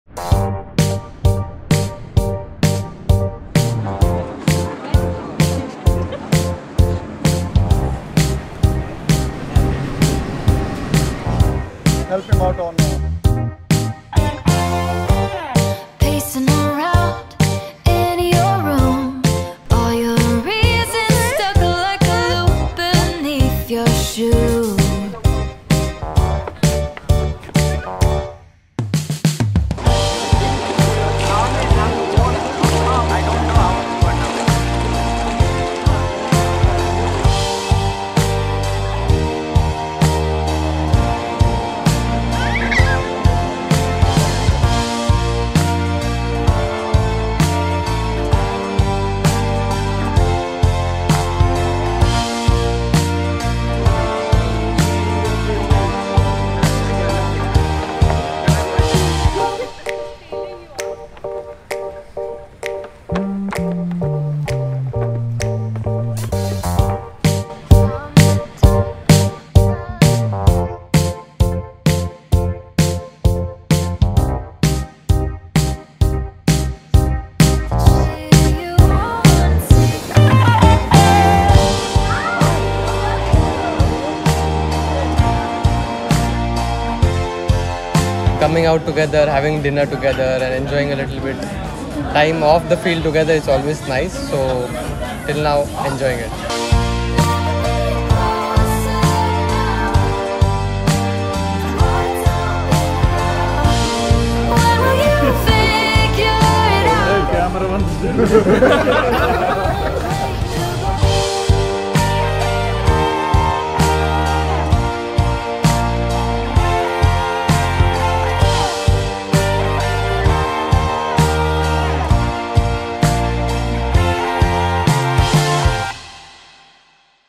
Supper time with the Super Fam! Whistles Kingdom 💛
The Super fam unwinds over dinner. Catch up all the fun and family moments as the Yellove makkal took some time off from the grind!